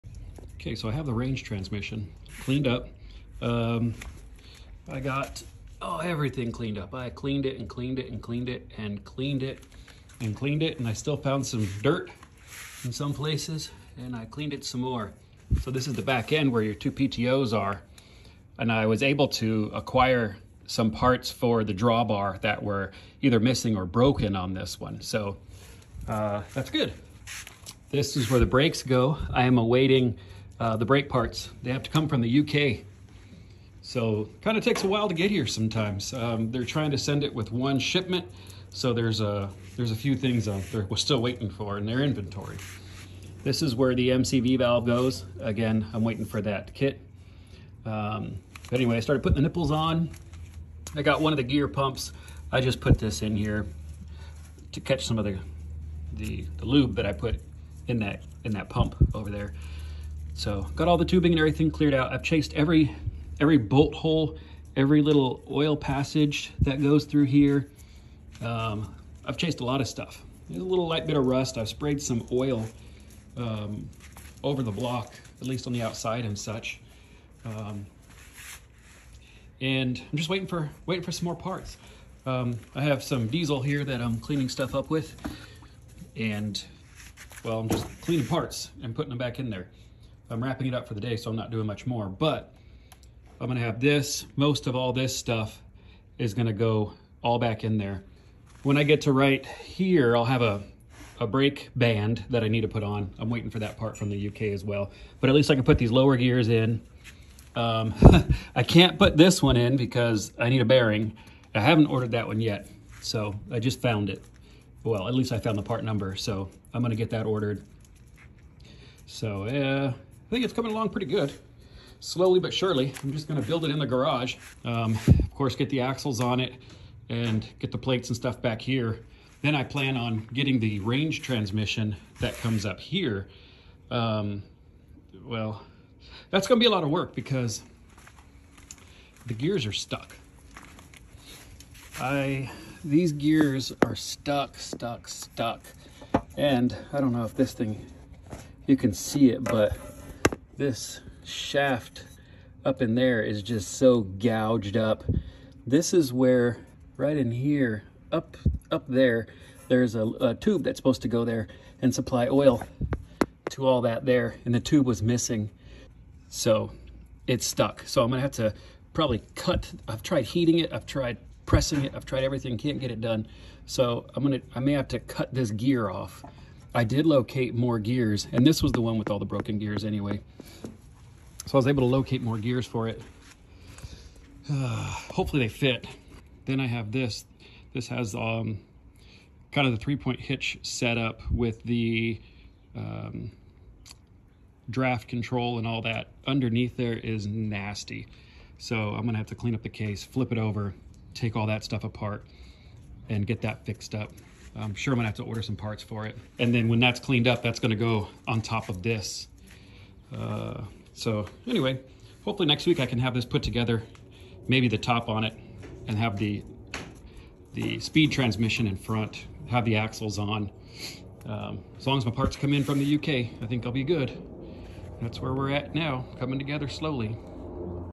Range trans Final touches before reassembly!
After a tremendous amount of work and parts acquisition, I'm finally to the point where I can reassemble this thing. Just a few final touches before reassembly.